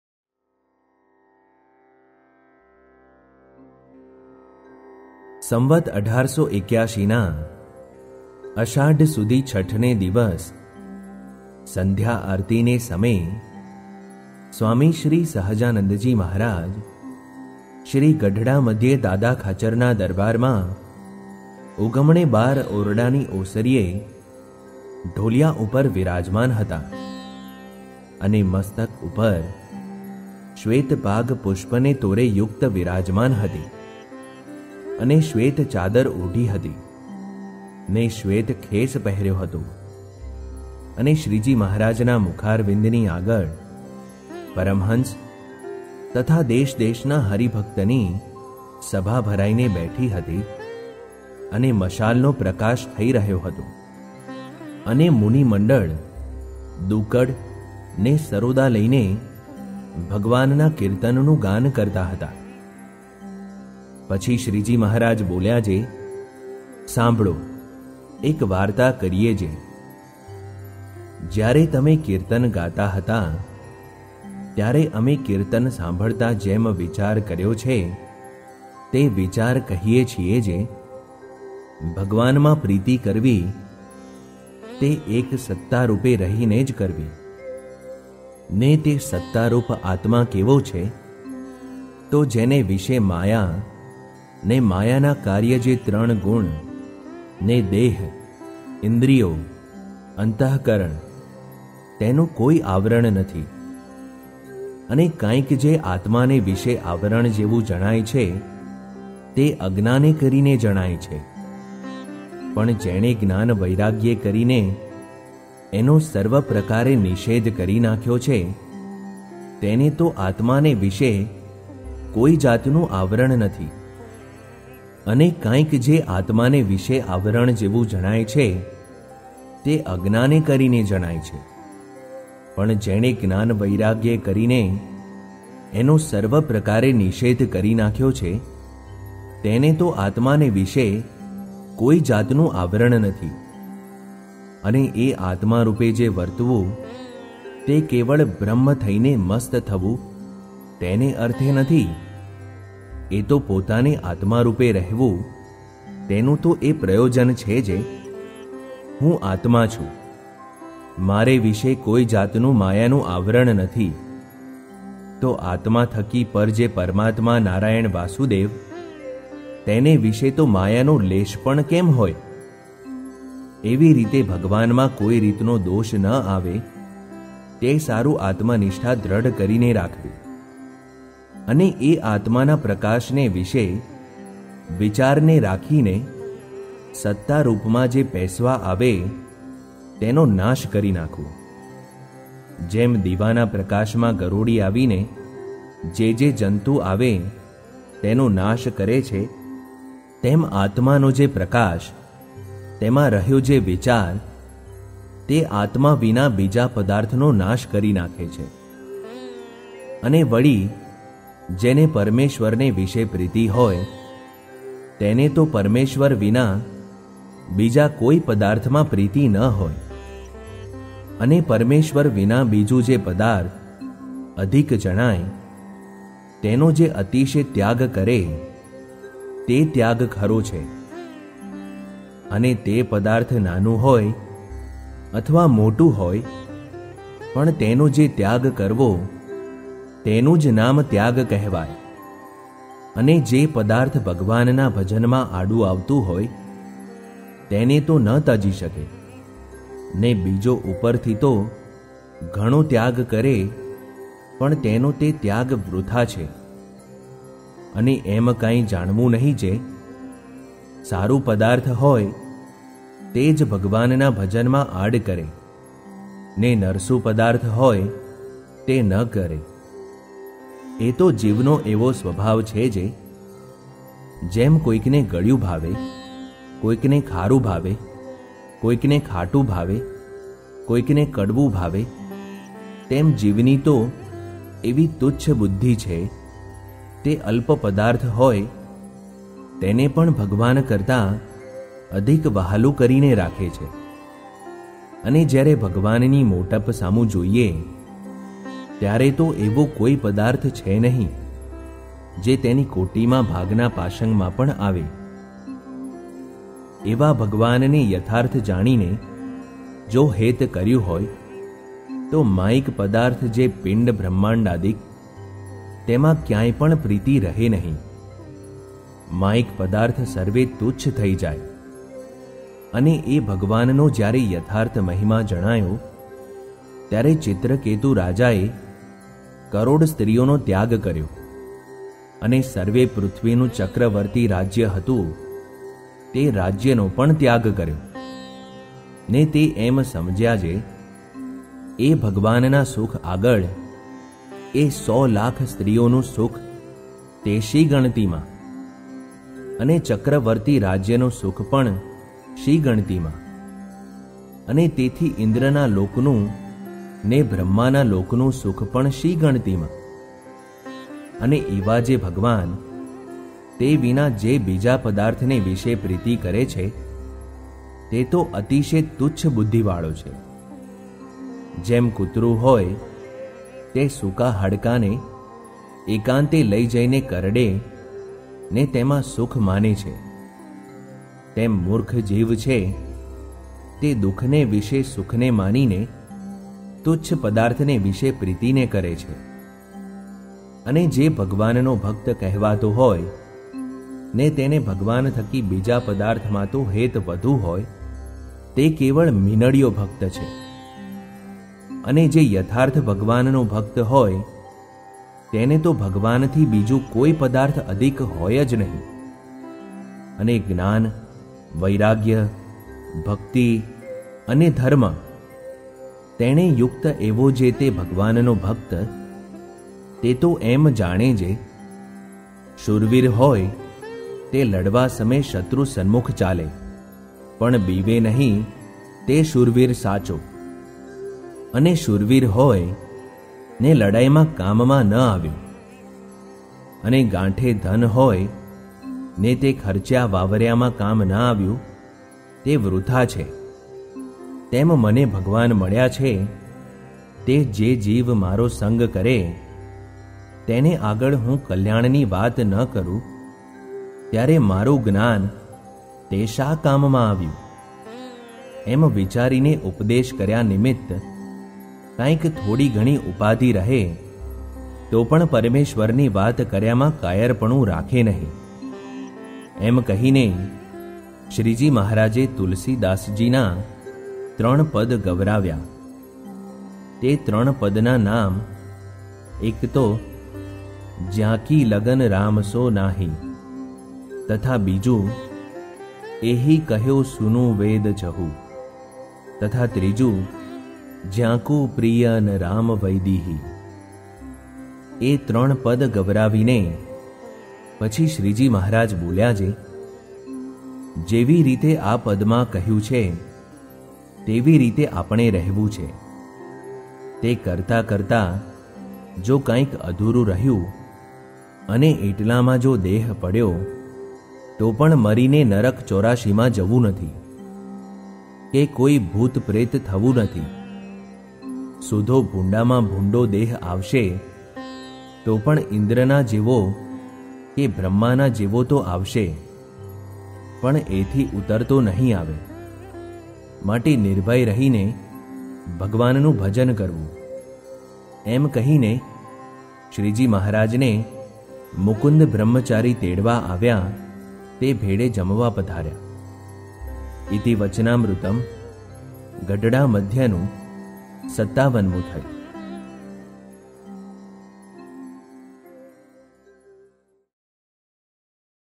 57 Gadhada Madhyam Vachanamrut || Audio Book
Vachanamrut - compilation of 273 spiritual discuss and discourses, which is divided into 10 sections. The discuss and discourses were delivered by Bhagwan Swaminarayan in the last decade of his life, between 1819 to 1829.
They were mostly delivered in in secluded places like Gadhada, Ahmedabad , Sarangpur, Kariyani, Loya, Panchala, Vadtal,Aslali and Jetalpur.
The editors for this were Muktanand Swami, Gopalanand Swami, Brahmanand Swami,Nityanand Swami and Shukanand Swami.